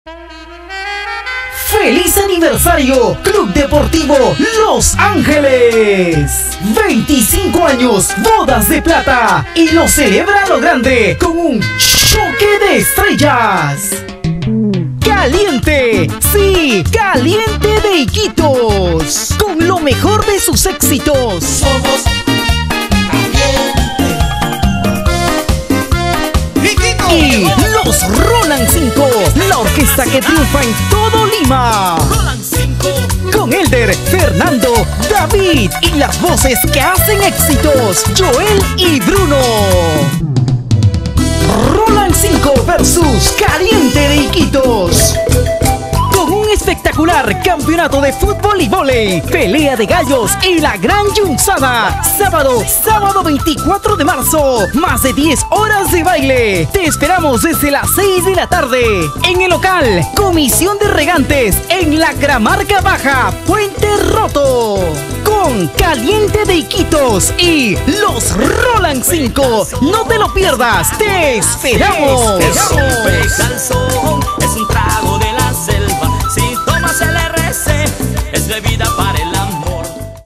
Rolans 5 vs Kaliente Club deportivo Los Angeles-Chimobote 24 marzo.
Feliz aniversario club deportivo los angeles ..que lo celebra con choque de estrellas,,Kaliente vs los Rolans 5 sabado 24 de marzo en el local lacramarca Baja - puente roto - chimbote desde las 6 de la tarde con campeonato de voley fulbito ..pelea de gallos y la gran yunsada...los esperamos.